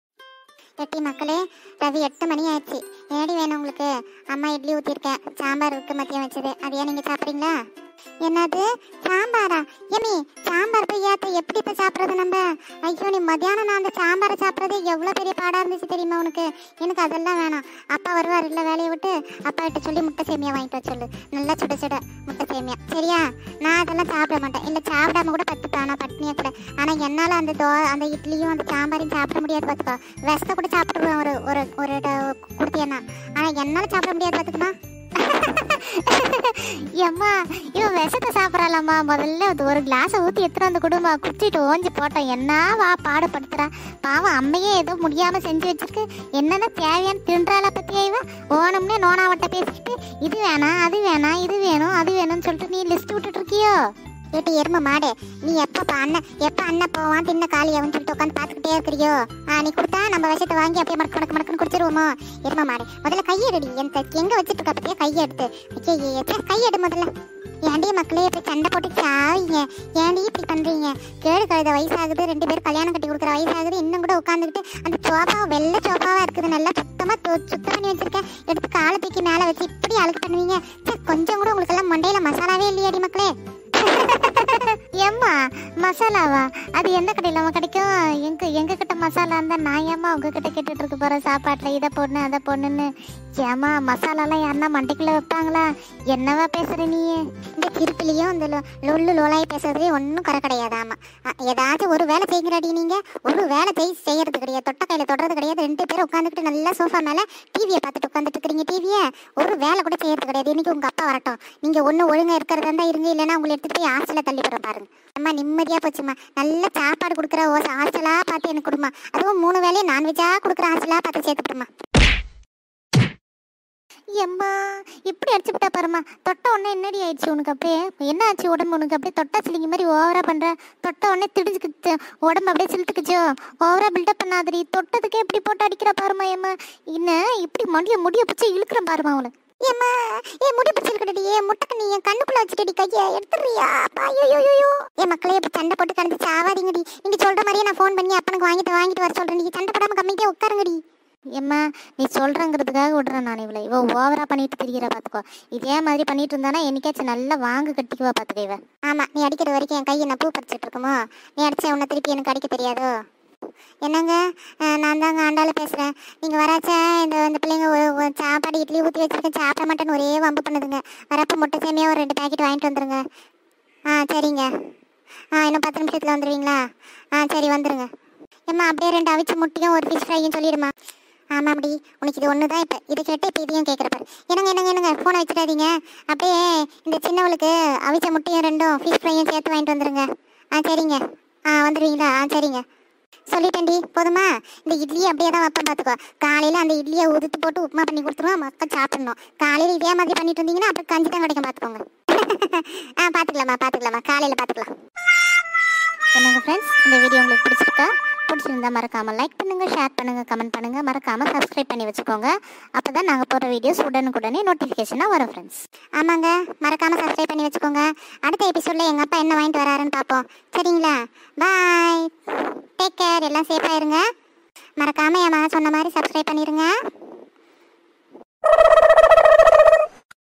🌹அம்மா பொண்ணு சண்டை 🤣😂யம்மா எனக்கு 🤣 முட்டசேமியா தா 🤣😂 வேணும் என்னால சாம்பார் இட்லி சாப்பிடமுடியாது
அம்மா பொண்ணு சண்டை.